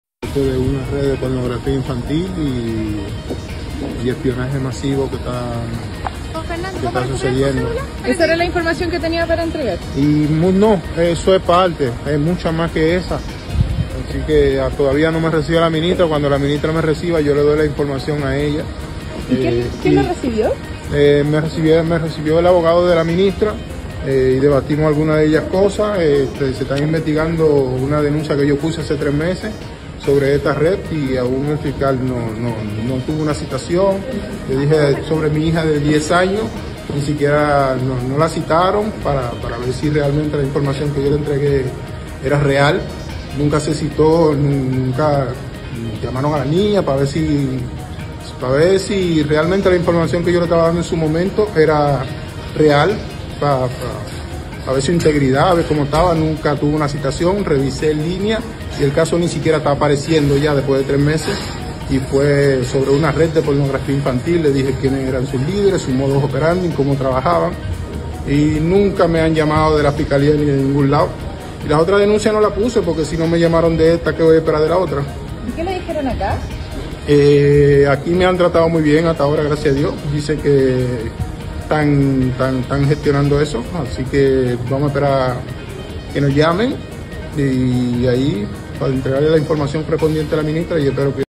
DENUNCIA DE RED DE PEDOFILIA Y ESPIONAJE.
Santiago Un hombre fue controlado por Carabineros tras pararse frente a La Moneda con un cartel: "Tengo información sensible que afecta la seguridad nacional"

Hombre asegura que cuenta con información de una red de pornografía infantil y espionaje masivo.